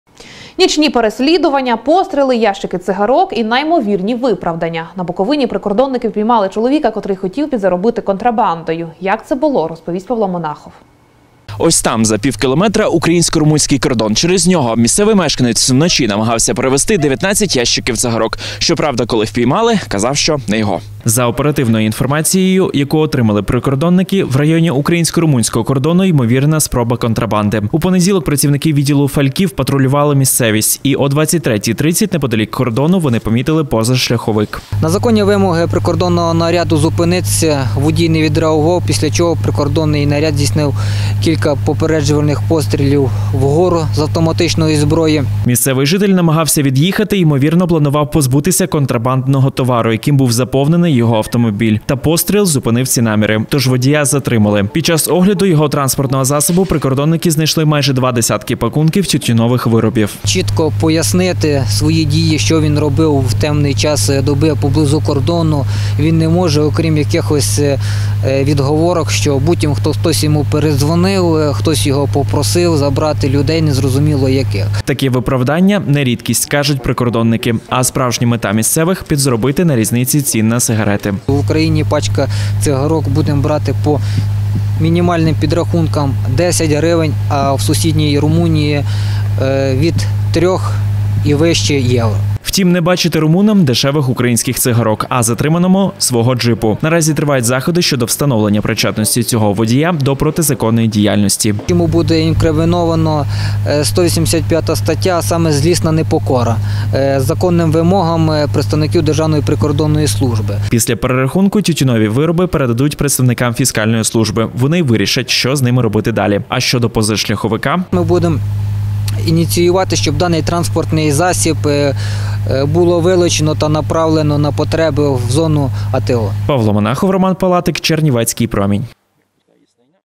Нічний рейс. З цигарками
160119 Буковинські прикордонники впіймали контрабандиста на джипі. Чоловік виправдовується: опівночі біля румунського кордону очікував… пасажирів.